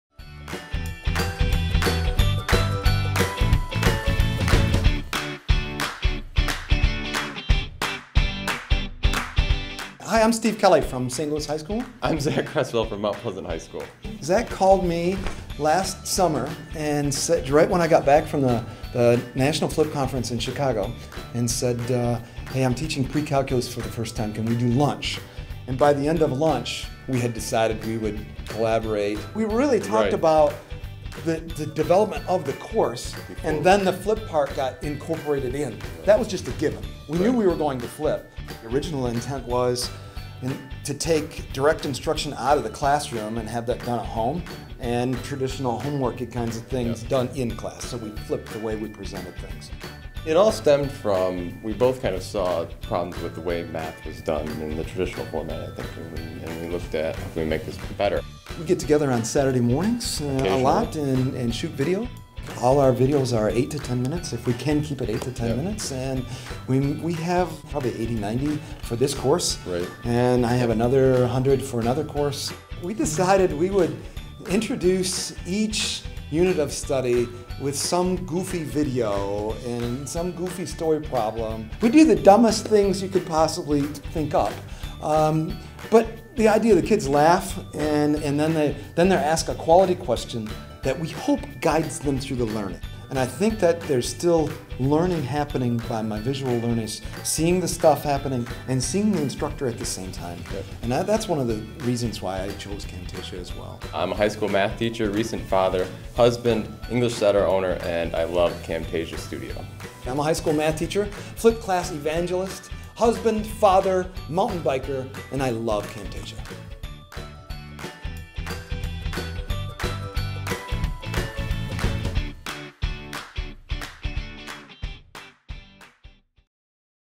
Michigan Teachers Team Up to Flip
Steve Kelly, of St. Louis High School, and Zach Cresswell, of Mt. Pleasant High School, have teamed up to flip their math classes together. Both teacher saw pitfalls with the traditional method of teaching math, so they got together to discuss course design and how to help their students learn more effectively. The solution they decided on was flipped learning. Instead of going their own route, Steve and Kelly put their heads together to create a curriculum that really resonated with both their classes.